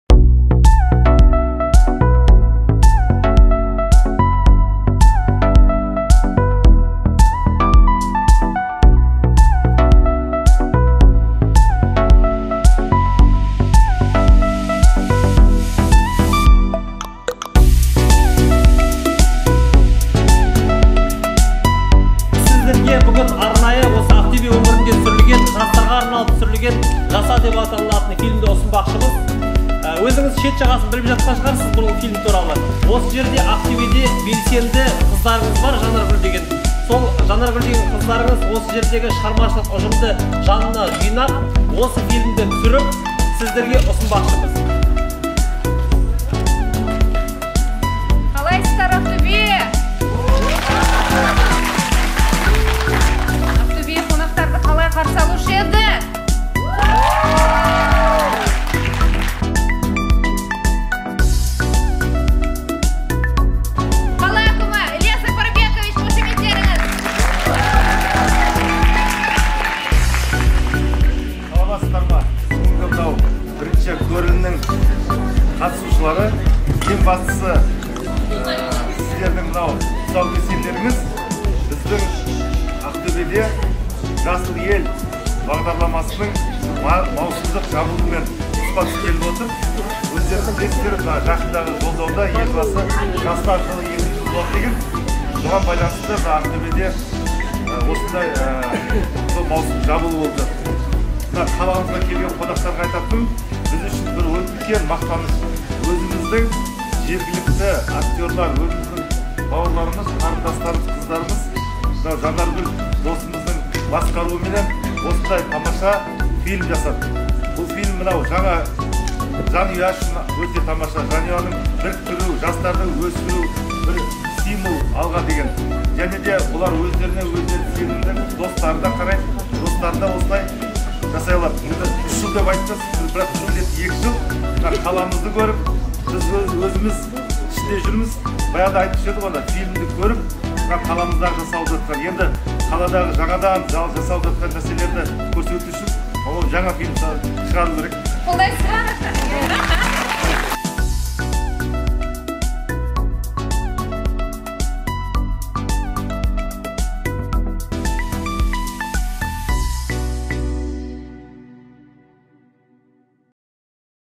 MEDIA PRO/ПРЕМЬЕРА ФИЛЬМА "ЖАСА"
Первый показ фильма состоялся в городе Актобе в кинотеатре "Локомотив" о том как прошло это мероприятие вы узнаете прямо сейчас из нашего видео репортажа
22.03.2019  вы сможете увидеть наш фильм на You Tube